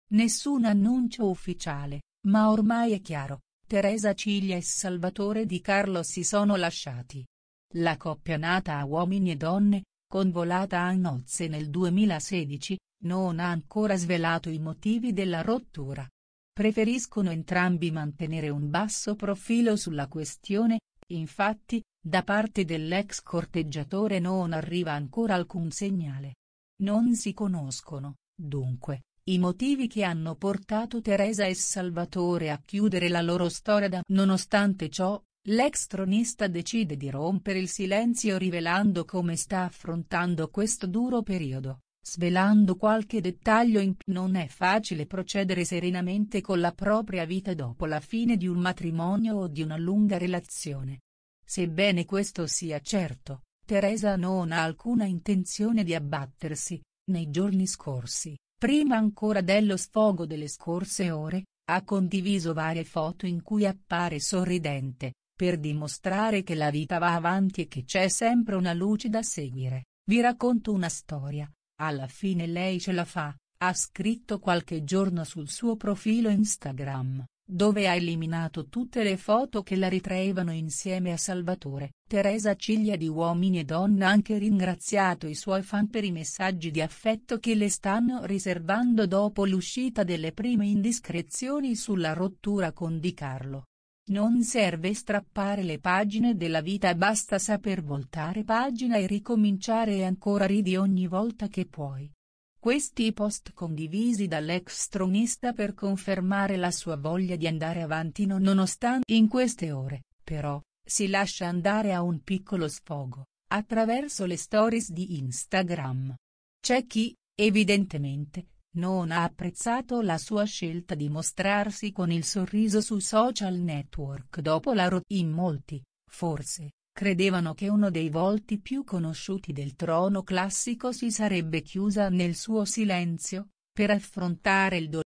UeD, Teresa e Salvatore è finita. Parla lei: “Sono stata malissimo”
Per la prima volta l’ex tronista entra nel dettaglio dopo la rottura con Di Carlo, lasciandosi andare a un piccolo sfogo